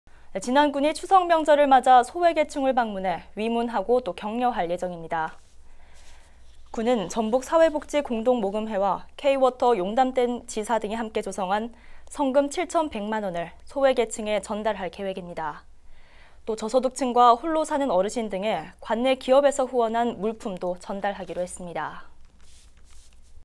진안군, '풍성한 한가위' 이웃 나눔 실천
진안군이 추석 명절을 맞아
소외계층을 방문해 위문하고 격려할 예정입니다.

군은 전북사회복지공동모금회와
K-water 용담댐지사 등이 함께 조성한
성금 7천100만 원을 소외계층에 전달할 계획입니다.

또 저소득층과 홀로 사는 어르신 등에
관내 기업에서 후원한 물품도 전달하기로 했습니다.